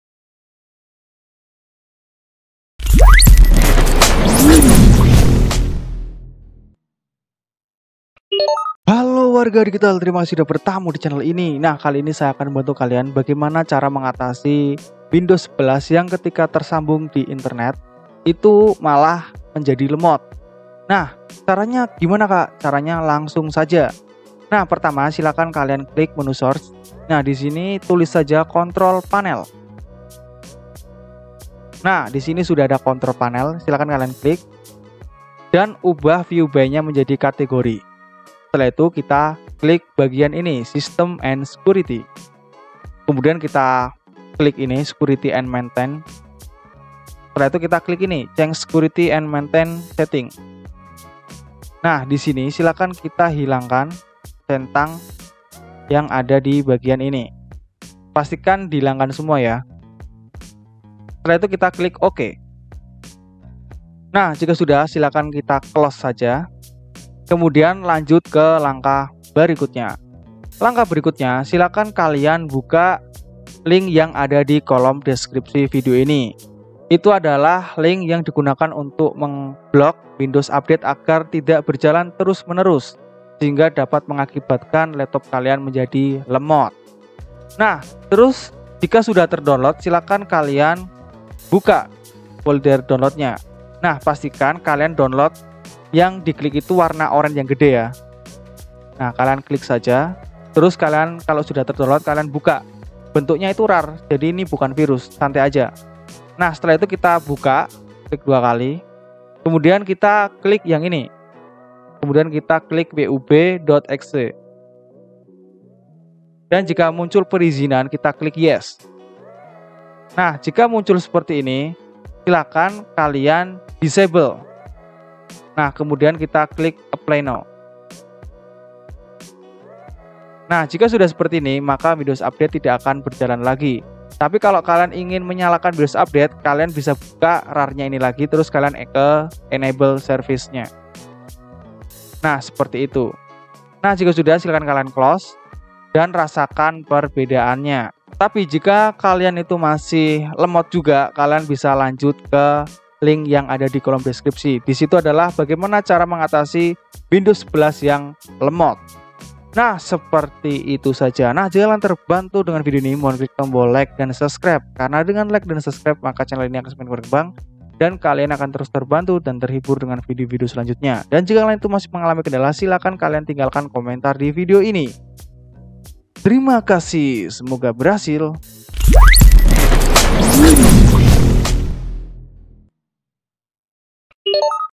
CARA MENGATASI WINDOWS 11/10 LEMOT SAAT TERHUBUNG ATAU TERKONEKSI KE INTERNET / WIFI - AMPUH BANGET
Semoga Kalian Tetap Sehat !
🔴Windows Update Blocker:
Sangat membantu untuk mengembangkan channel ini heheh 😁